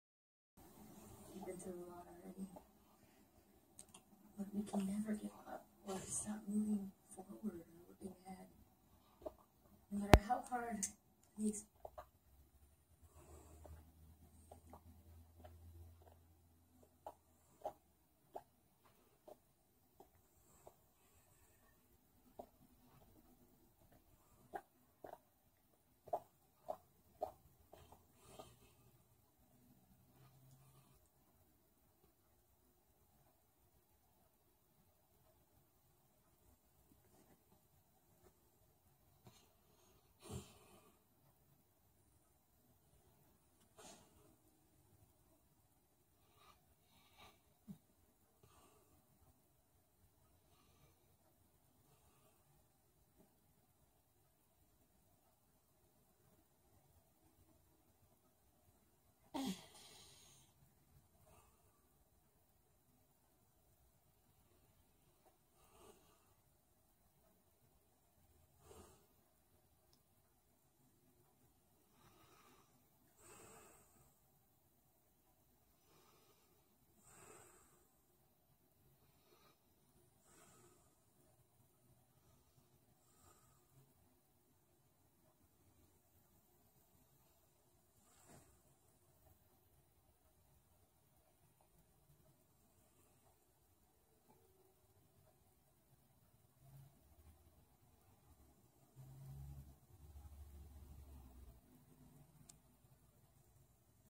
Labrador Moby dreaming about suckling teat
Look at his cute little 😜 tongue!